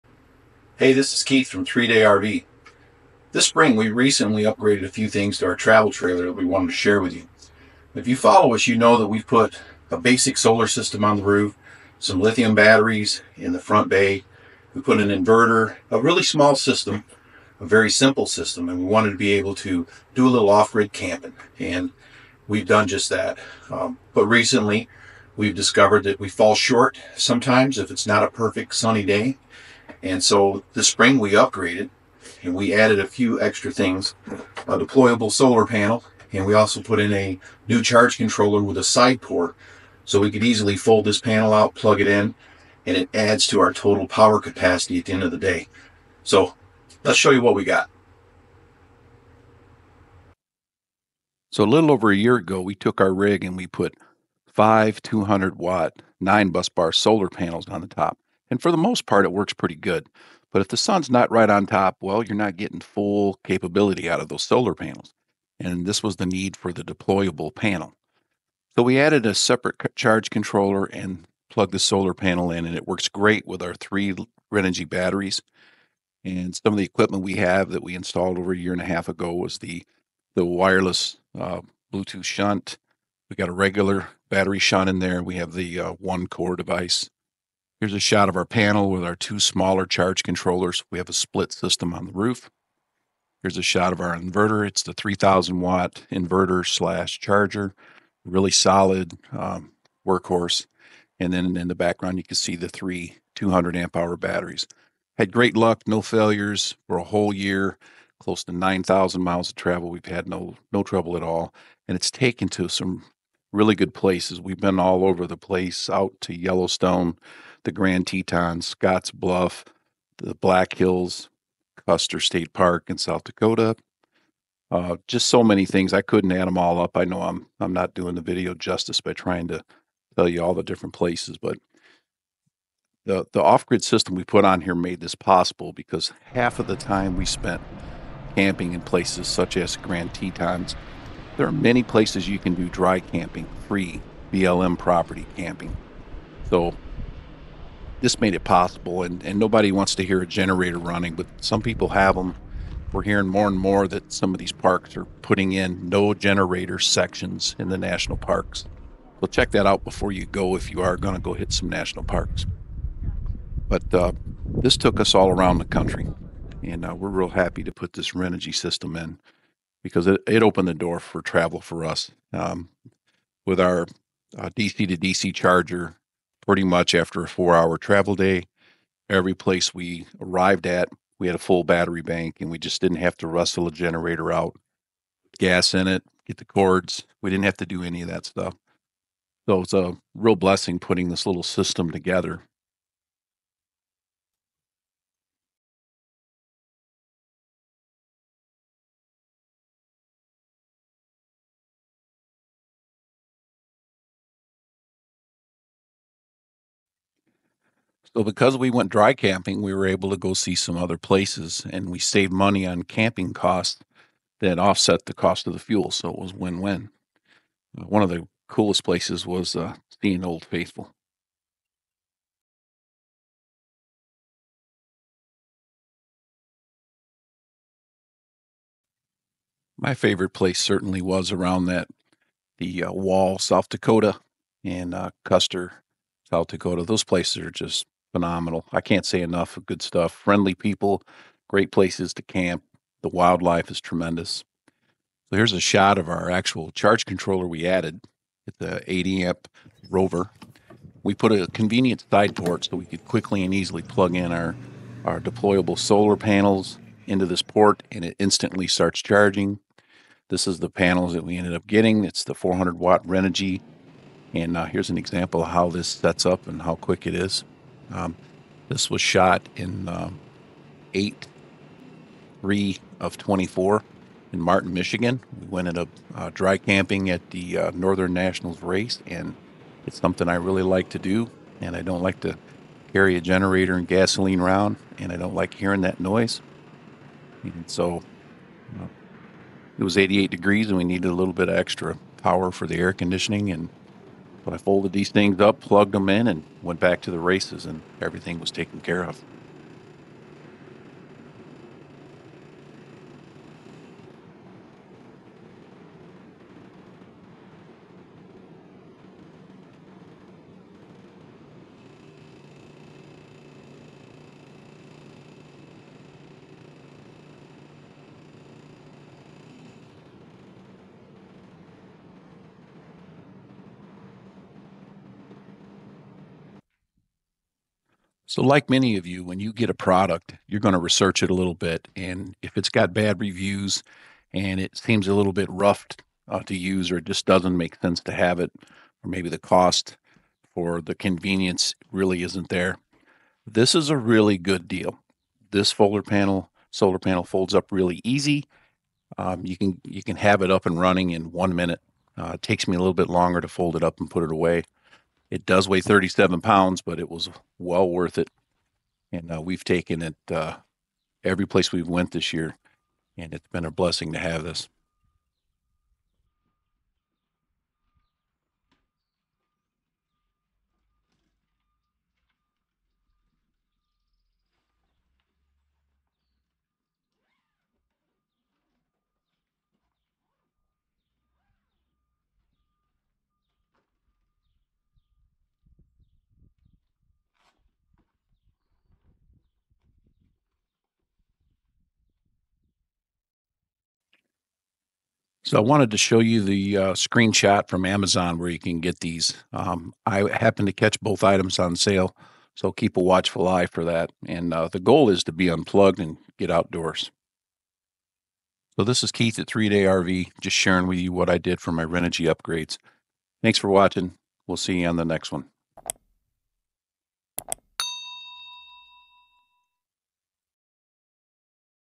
Renogy Experience Upgrade Part II
This is a video review of how our Renogy experience went as we added to our offgrid solar/power system in our RV.

It is simply amazing how much of a difference a portable panel can be as the sun is just coming up or going down for the evening.

We added the 400W Renogy foldable solar panel, a 80A Rover (for future expansion) and use a SAE quick port that is on 10AWG wire.

So far we have had fantastic luck with all of our Renogy products and it helps our RV really feel much like home as we explore the sites across this beautiful country.

3 x 200 Ah Renogy Core Batteries
3Kw inverter charger by Renogy
1) 660 W MPPT DC to DC charge controller

1) Rover 40 amp MPPT charge controller
1) Rover 60 amp MPPT charge controller
5) 9 BB 200 W solar panels
1-500 amp Renogy Shunt
1- 300 to 500A Renogy Wireless shunt
1) One Core gateway

2022 Coachmen Freedom Express 259FKDS

If you are interested, below is the video to how it all started a year and a half ago, we covered the planning, the purchase and the install:

Never enough time!